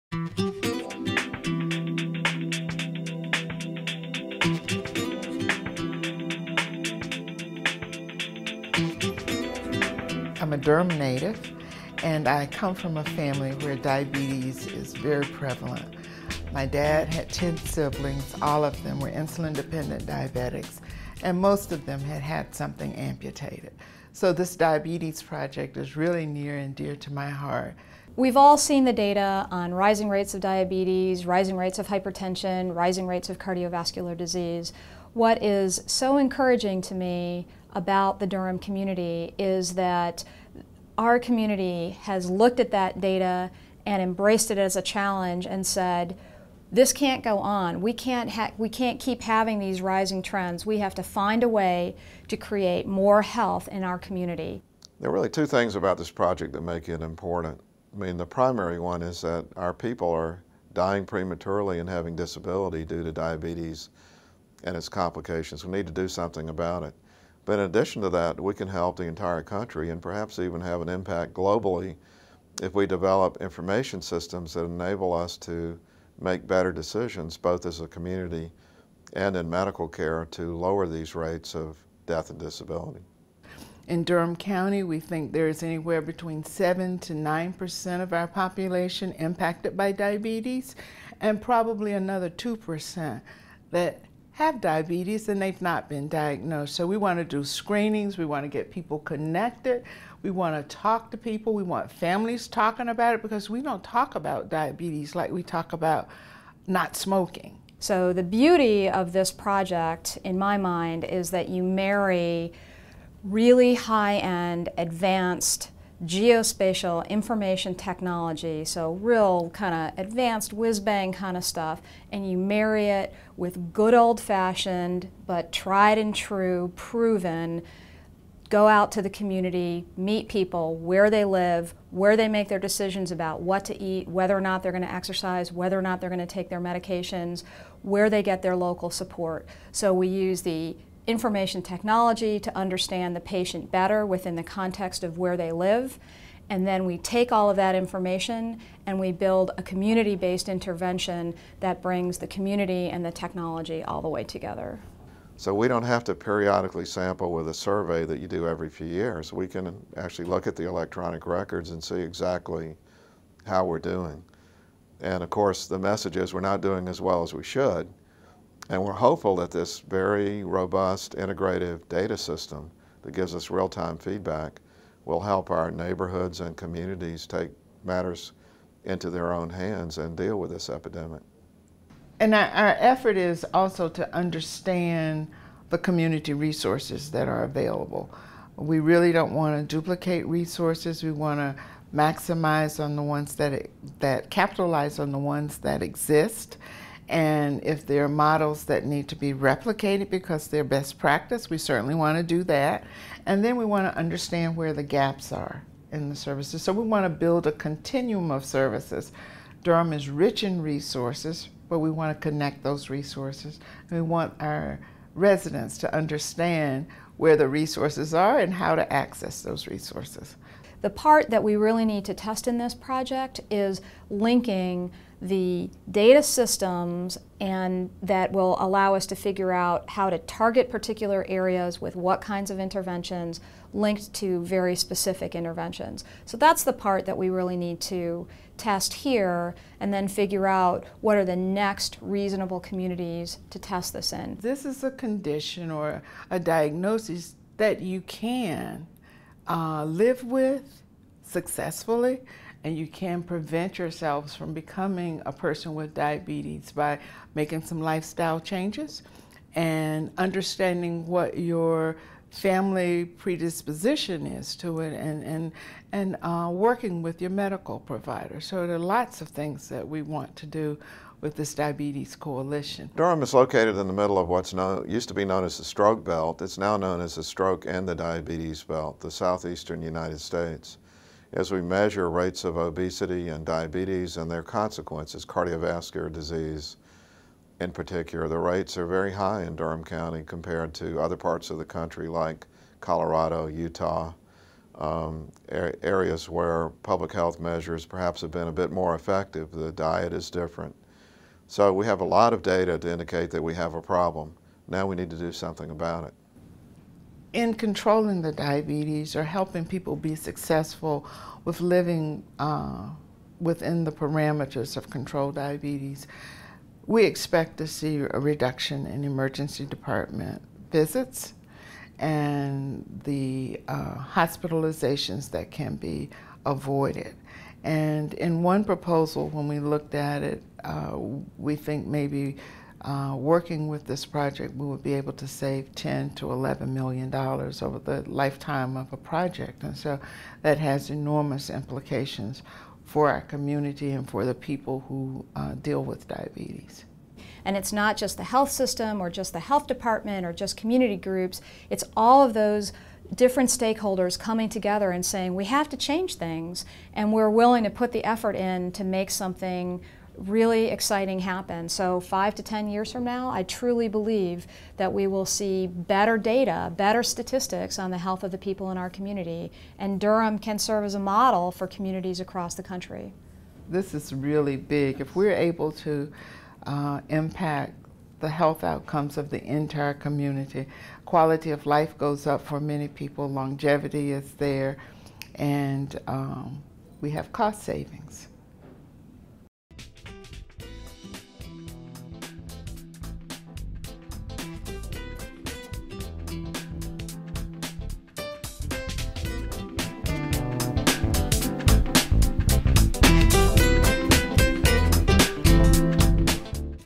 Coalition I
Durham County Public Health Director Gayle Harris, Marie Lynn Miranda, PhD with the University of Michigan, and Dr. Robert Califf with the Duke University Medical Center, talk about ways the Durham Diabetes Coalition will help the community.